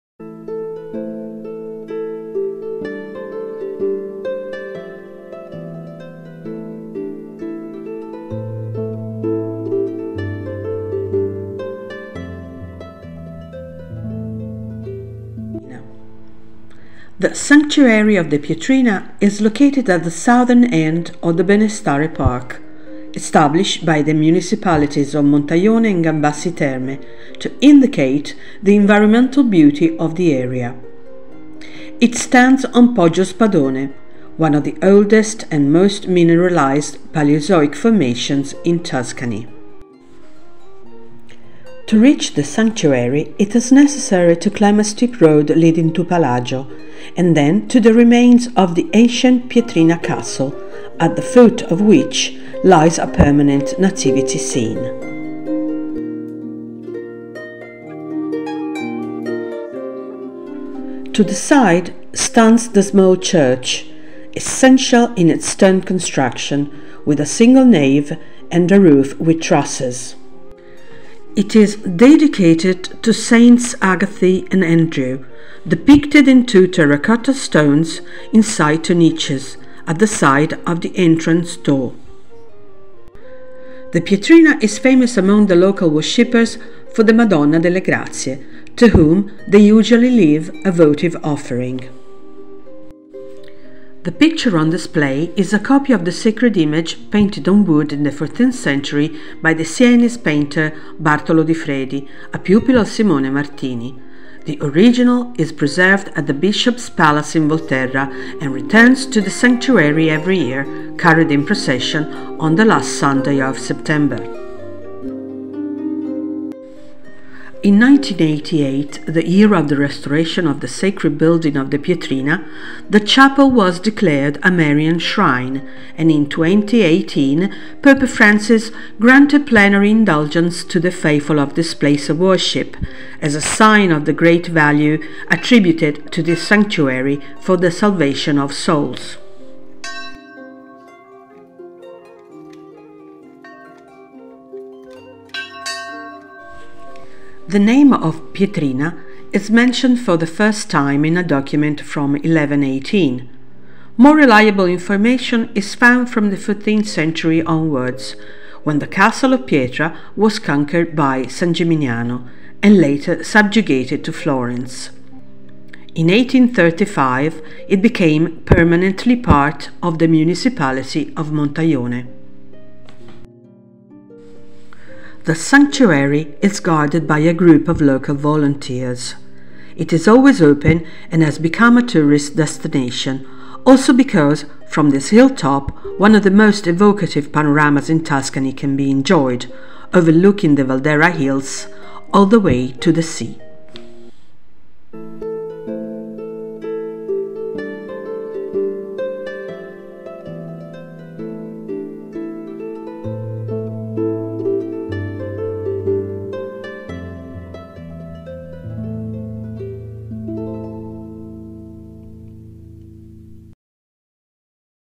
THE SANCTUARY OF THE PIETRINA, Montaione, Italy - English Version
THE SANCTUARY OF THE PIETRINA

The Sanctuary of the Pietrina is located at the southern end of the Benestare Park, established by the municipalities of Montaione and Gambassi Terme to indicate the environmental beauty of the area. It stands on Poggio Spadone, one of the oldest and most mineralised Palaeozoic formations in Tuscany.
To reach the sanctuary, it is necessary to climb a steep road leading to Palagio and then to the remains of the ancient Pietrina Castle, at the foot of which lies a permanent nativity scene.
To the side, stands the small church, essential in its stone construction, with a single nave and a roof with trusses.
It is dedicated to Saints Agatha and Andrew, depicted in two terracotta statues inside two niches, at the side of the entrance door.
The Pietrina is famous among the local worshippers for the Madonna delle Grazie, to whom they usually leave a votive offering.
The picture on display is a copy of the sacred image, painted on wood in the 14th century by the Siénese painter Bartolo di Fredi, a pupil of Simone Martini; the original is preserved at the Bishop's Palace in Volterra and returns to the sanctuary every year, carried in procession, on the last Sunday of September.
In 1988, the year of the restoration of the sacred building of the 'Pietrina', the chapel was declared a 'Marian Shrine', and in 2018 Pope Francis granted plenary indulgence to the faithful of this place of worship, as a sign of the great value attributed to this sanctuary for the salvation of souls.
The name of Pietrina is mentioned for the first time in a document from 1118; more reliable information is found from the 14th century onwards, when the castle of Pietra was conquered by San Gimignano, and later subjugated to Florence. In 1835 it became permanently part of the municipality of Montaione.
The sanctuary is guarded by a group of local volunteers. It is always open and has become a tourist destination, also because from this hilltop one of the most evocative panoramas in Tuscany can be enjoyed, overlooking the Valdera hills, all the way to the sea.